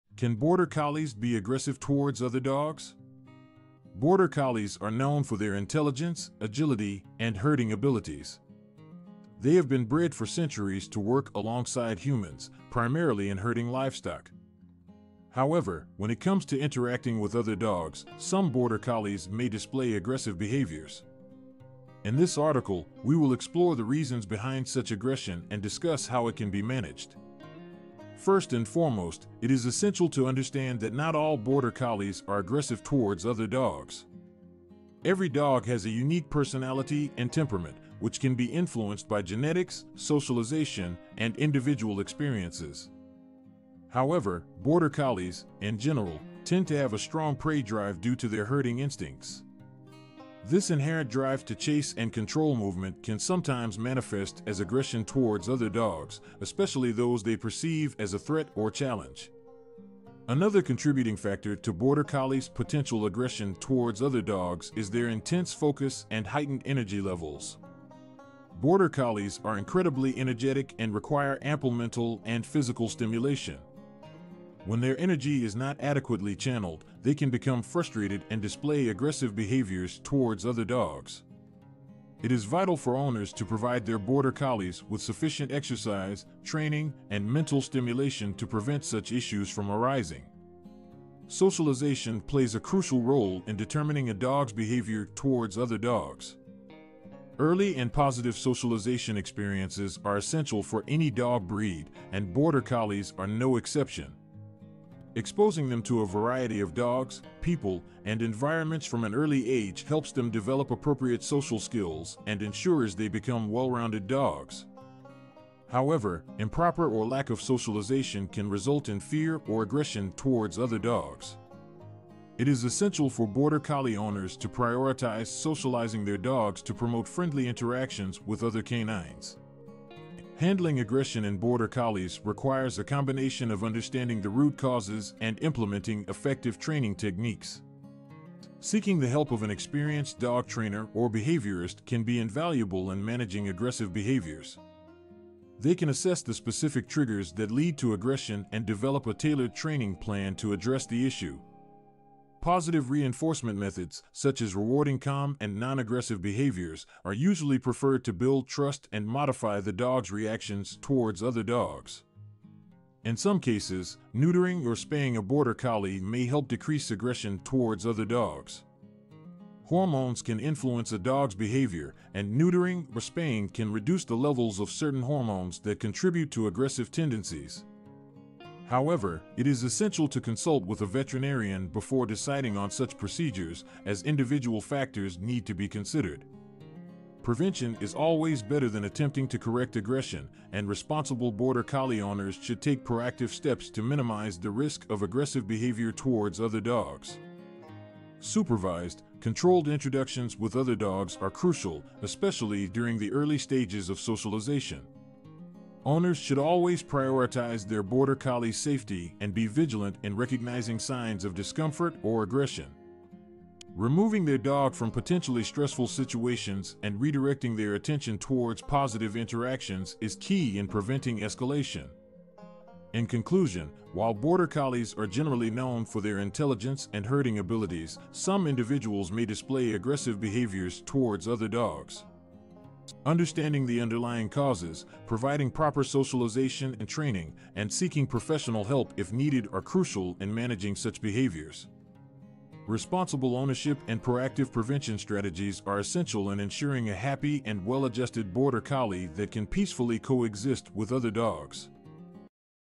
Can Border Collies be Aggressive Towards Other Dogs?
Border Collies are known for their intelligence, agility, and herding abilities
  They have been bred for centuries to work alongside humans, primarily in herding livestock
  However, when it comes to interacting with other dogs, some Border Collies may display aggressive behaviors
  In this article, we will explore the reasons behind such aggression and discuss how it can be managed
First and foremost, it is essential to understand that not all Border Collies are aggressive towards other dogs
  Every dog has a unique personality and temperament, which can be influenced by genetics, socialization, and individual experiences
  However, Border Collies, in general, tend to have a strong prey drive due to their herding instincts
  This inherent drive to chase and control movement can sometimes manifest as aggression towards other dogs, especially those they perceive as a threat or challenge
Another contributing factor to Border Collies' potential aggression towards other dogs is their intense focus and heightened energy levels
  Border Collies are incredibly energetic and require ample mental and physical stimulation
  When their energy is not adequately channeled, they can become frustrated and display aggressive behaviors towards other dogs
  It is vital for owners to provide their Border Collies with sufficient exercise, training, and mental stimulation to prevent such issues from arising
Socialization plays a crucial role in determining a dog's behavior towards other dogs
  Early and positive socialization experiences are essential for any dog breed, and Border Collies are no exception
  Exposing them to a variety of dogs, people, and environments from an early age helps them develop appropriate social skills and ensures they become well-rounded dogs
  However, improper or lack of socialization can result in fear or aggression towards other dogs